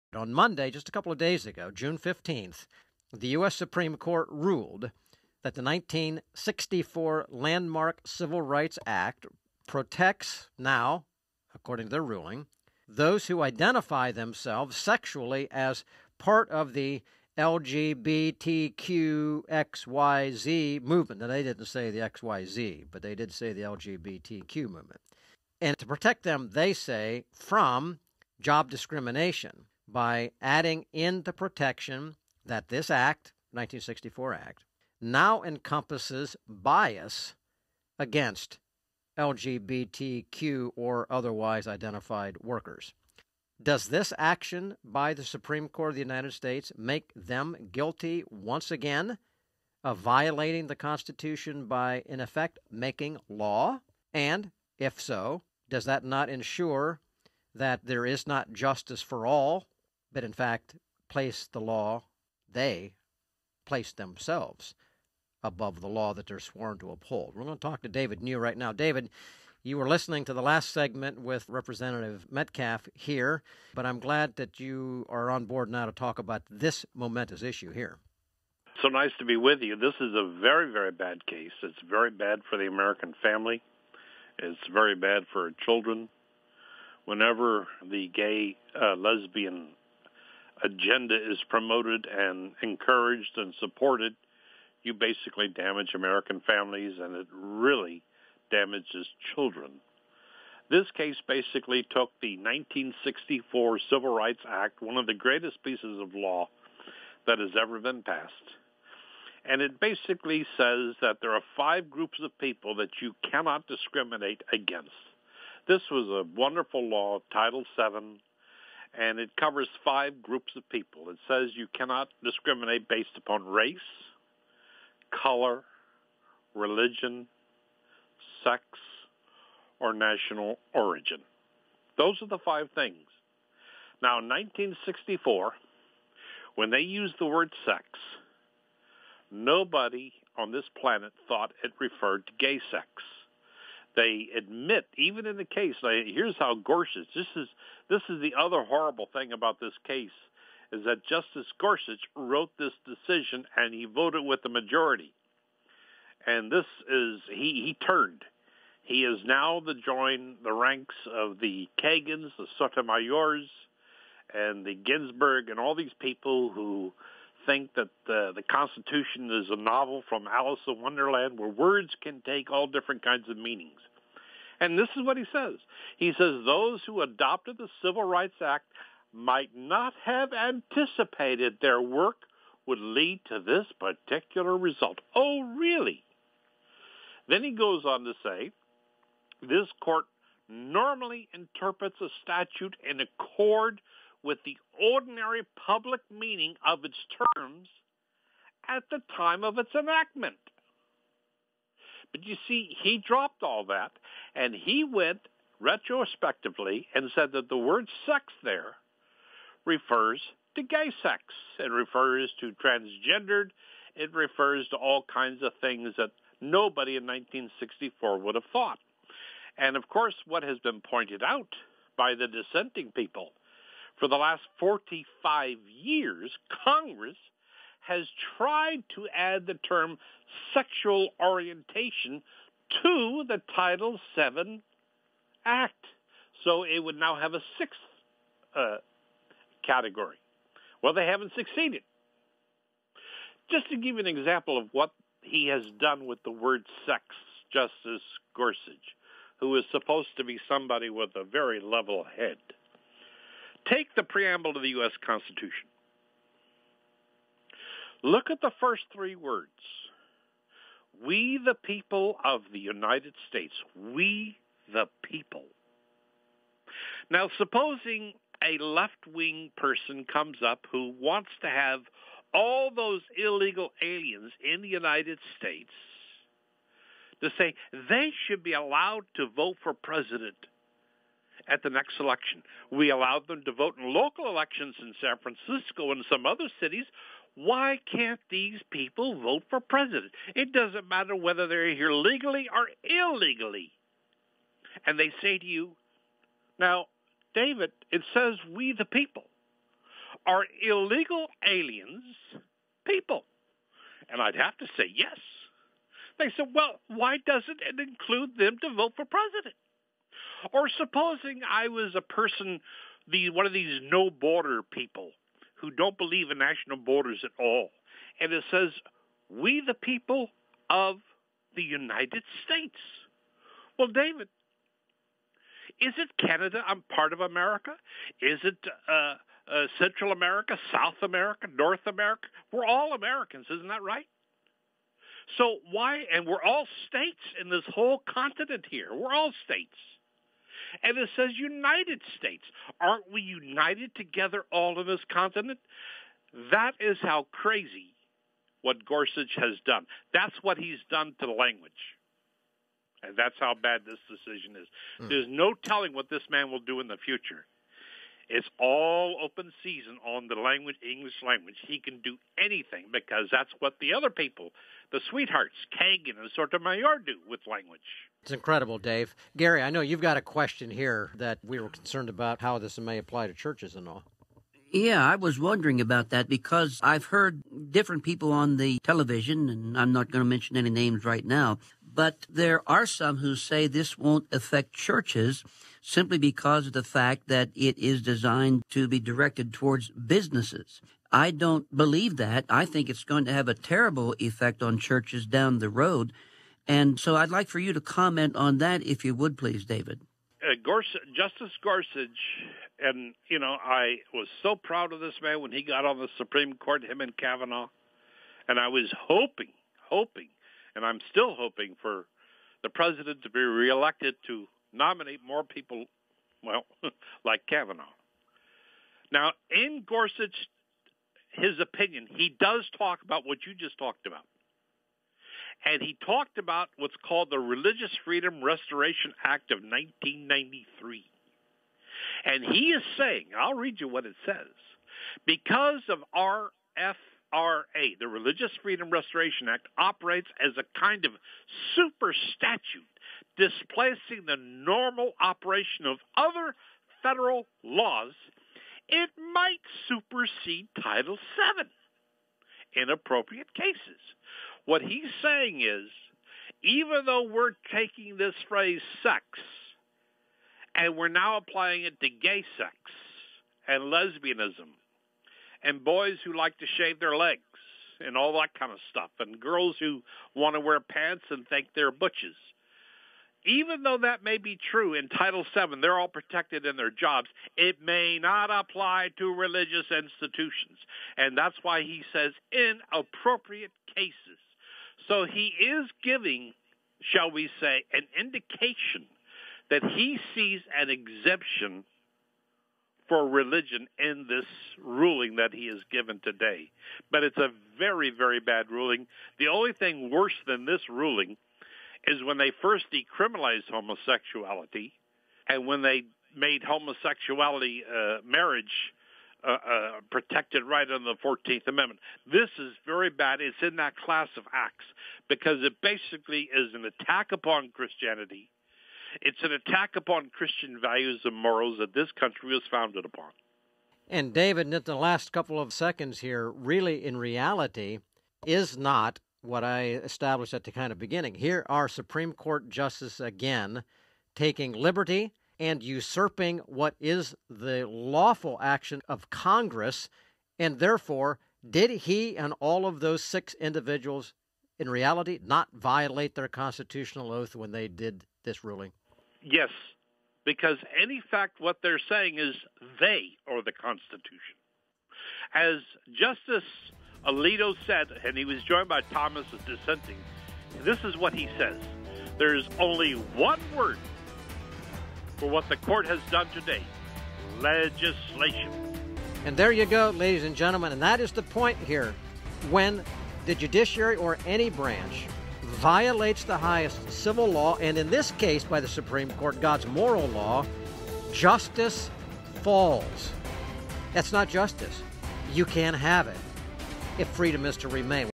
SCOTUS Violates the Constitution - Makes Law Again
On Monday June 15, 2020 the U.S. Supreme Court ruled that the 1964 landmark Civil Rights Act protects those who identify themselves sexually as part of the LGBTQ movement from what is referred to as ‘job discrimination’ by adding in the protection that this Act now encompasses bias against LGBTQ workers.

Does this action by the SCOTUS make them guilty once again of violating the Constitution by ‘making law’ and not ensuring “Justice for All” by placing themselves above the law they are sworn to uphold?

How might this decision impact churches and religious organizations?

Speakers: Sam Rohrer, David New, Gary Dull